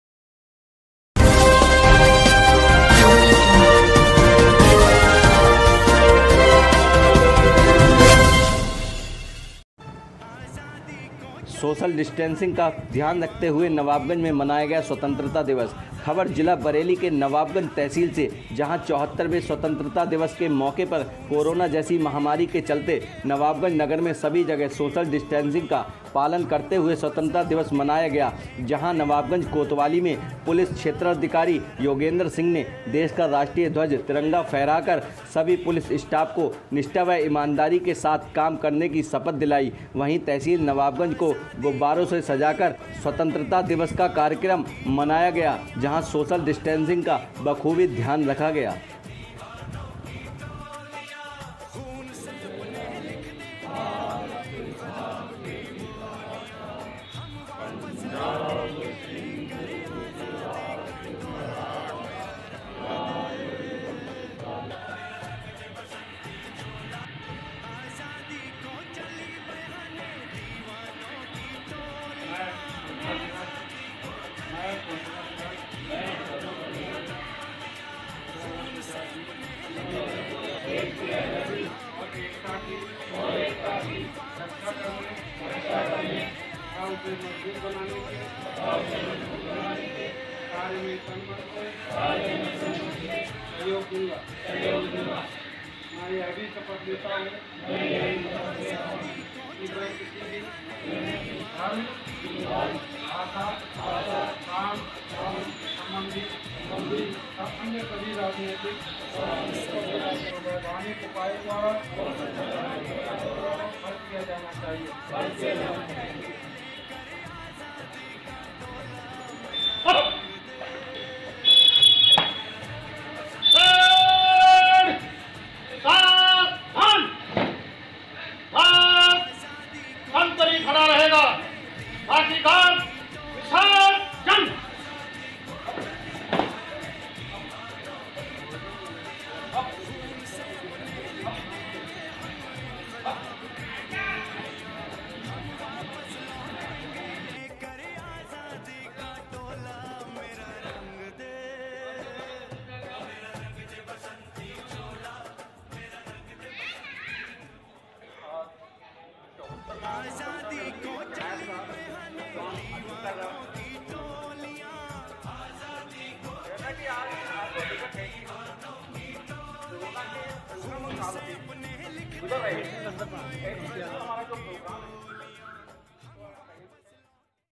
LIVE BHARAT TVनवाबगंज में सोशल डिस्टेंसिंग का ध्यान रखते हुए मनाया गया स्वतंत्रता दिवस
Electronic media network uttar Pradesh uttrakhand